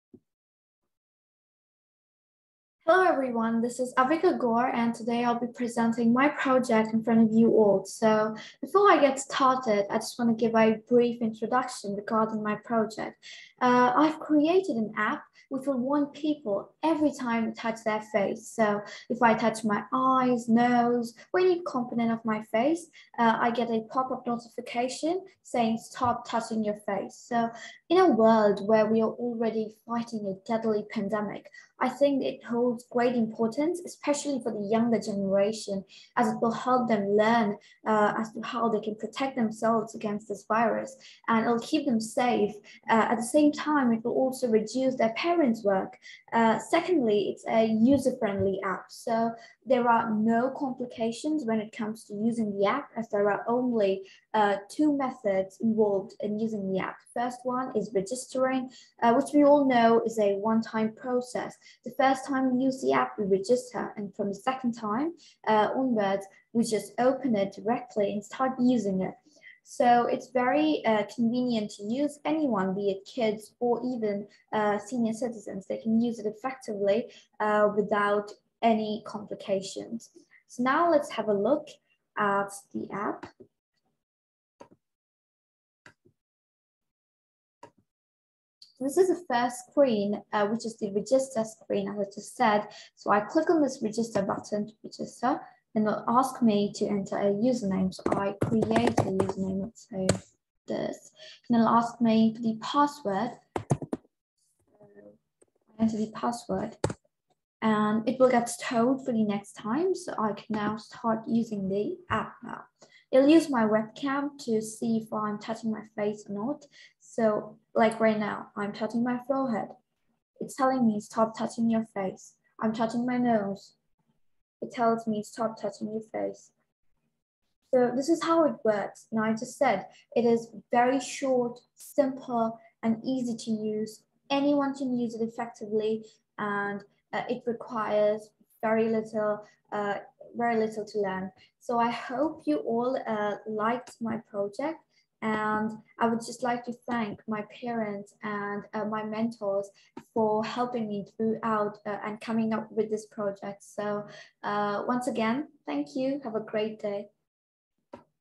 Don't touch your face by Avika Gaur | AIIP January 2022 cohort | Clevered
Avika Gaur created a project named Don't touch your face in the Artificial Intelligence Internship Program (AIIP) January 2022 cohort. It uses Intelligent  Agents, Pose recognition to warn people when they touch their face. The objective of this project is to help people to avoid touching their faces especially during this pandemic situation.